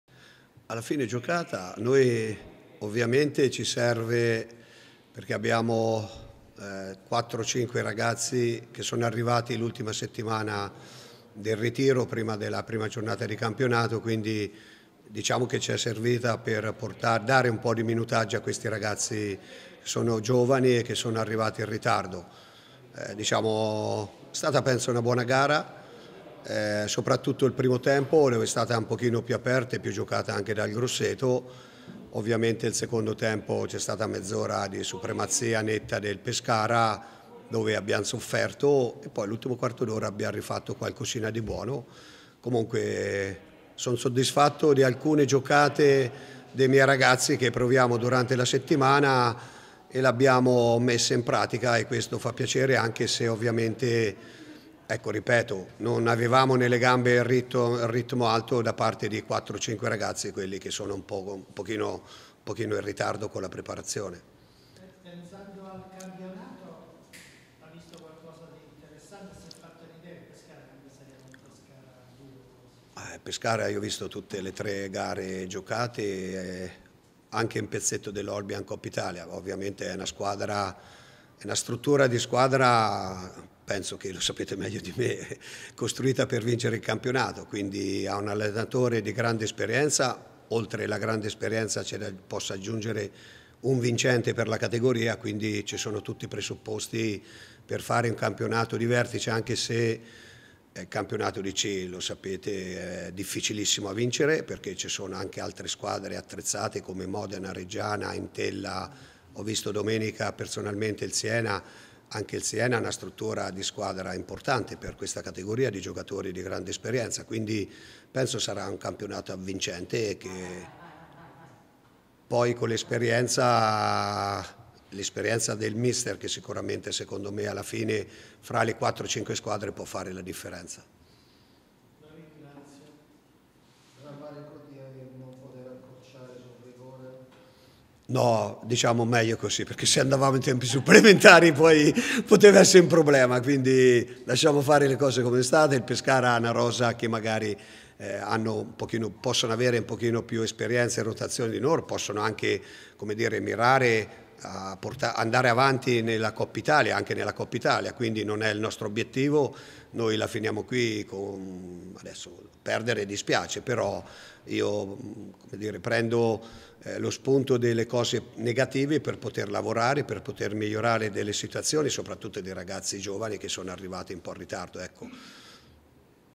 Coppa Italia. Pescara - Grosseto 2-0: Lamberto Magrini
Conferenza post gara di coppa dell'allenatore del Grosseto